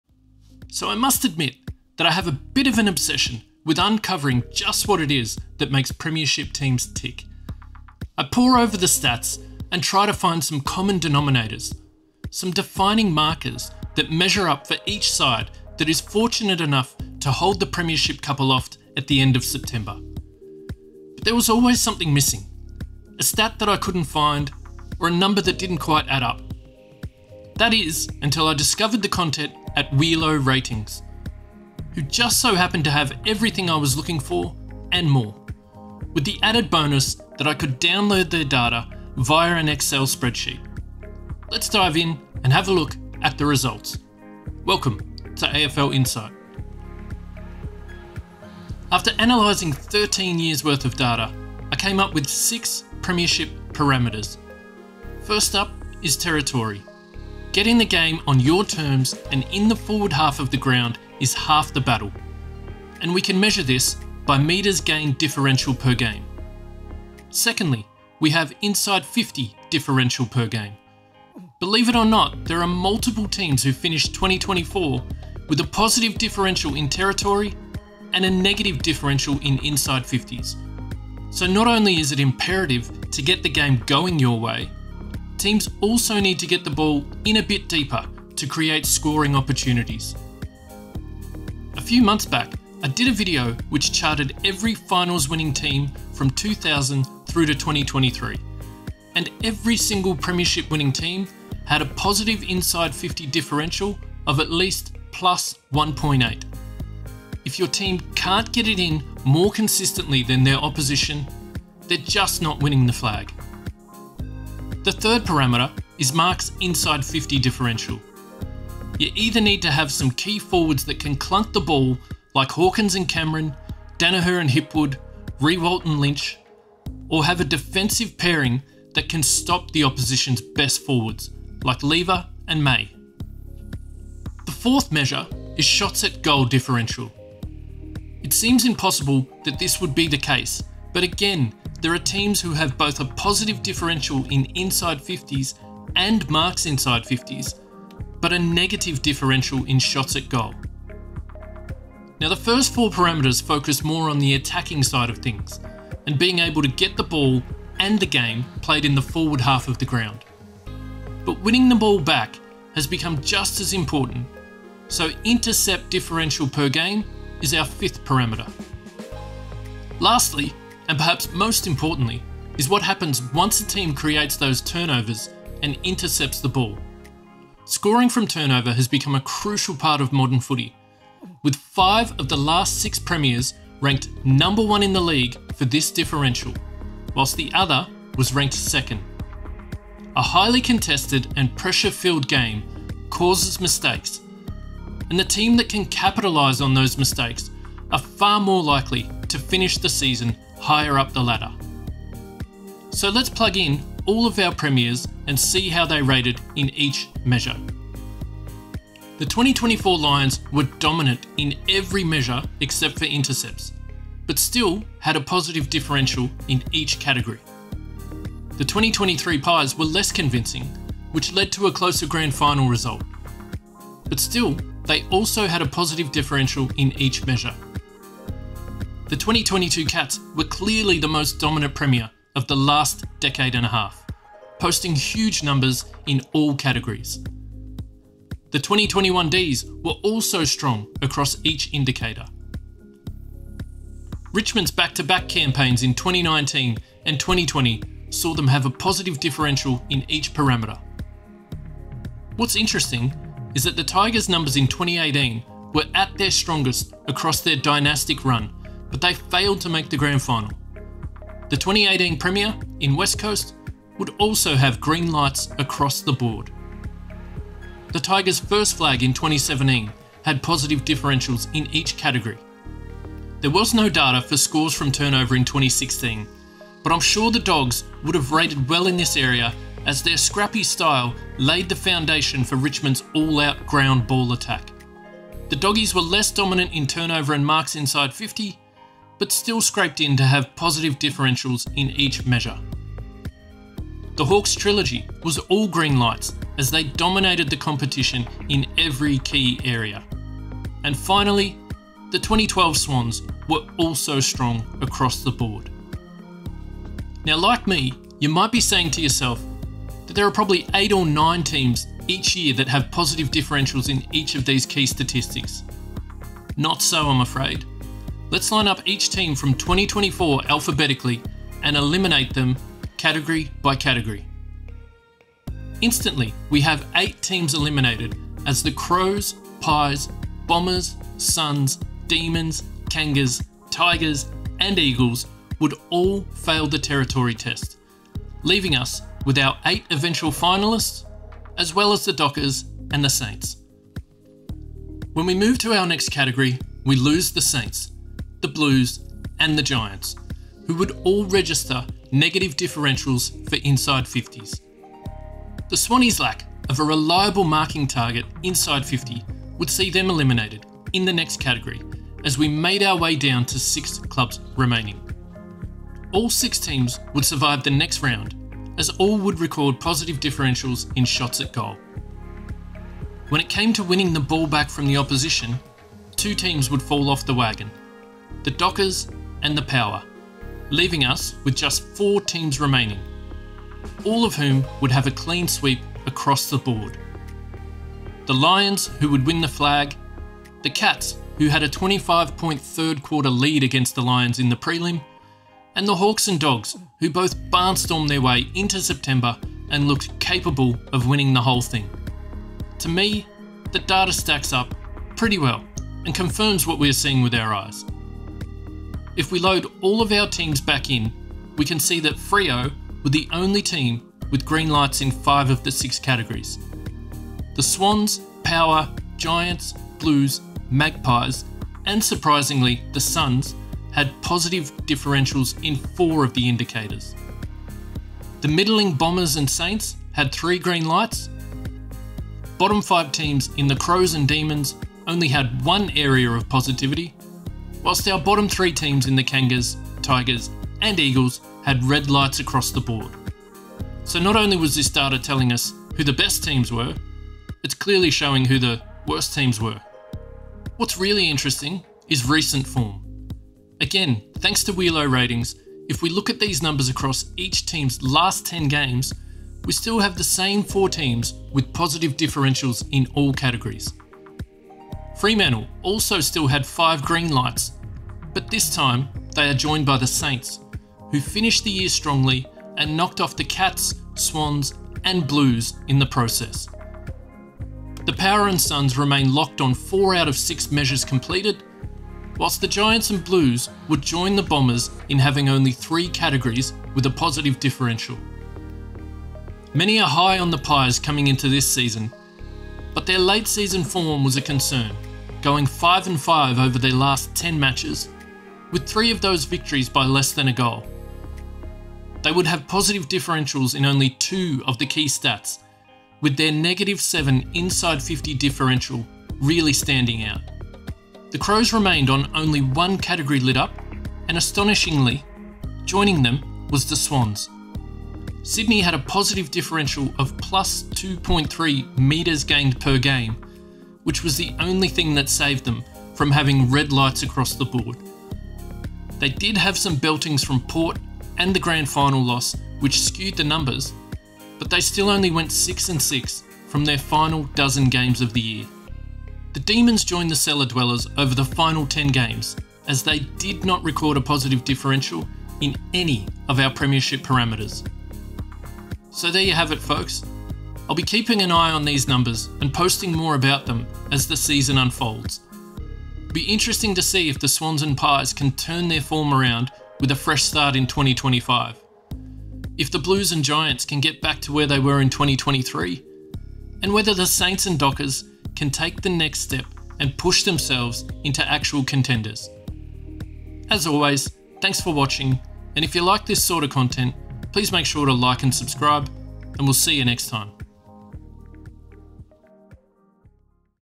Premiership Parameters: The Data Driving Modern Premiers
After analyzing 13 years worth of data, I have identified six definitive statistical categories that each Premiership Team had positive differentials in. Join us as we go through each KPI and see where your team sits in the pecking order.